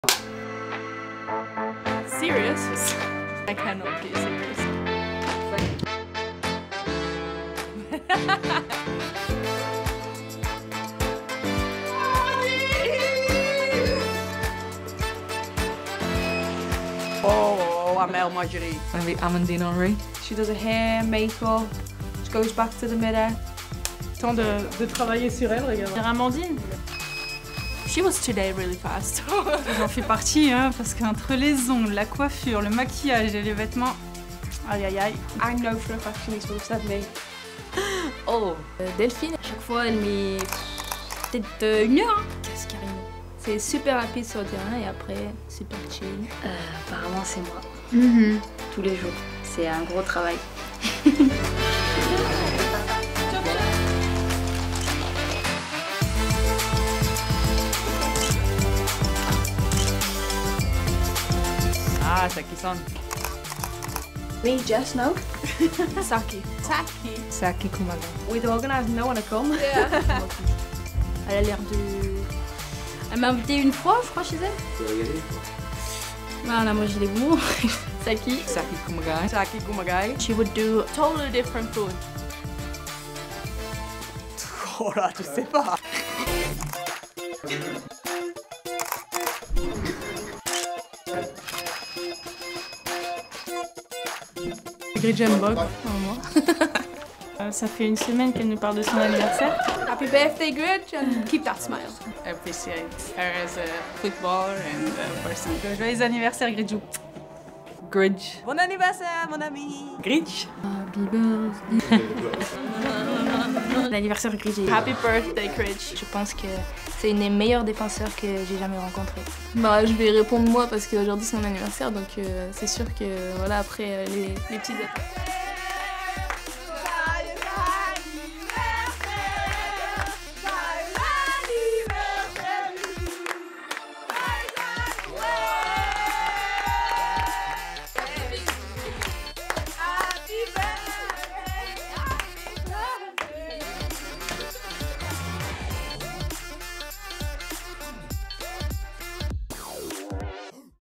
MOST LIKELY TO with the UWCL CHAMPIONS LYON!!
Meet the stars of Lyon's UEFA Women's Champions League team, as they are asked to name which teammate is 'most likely to'...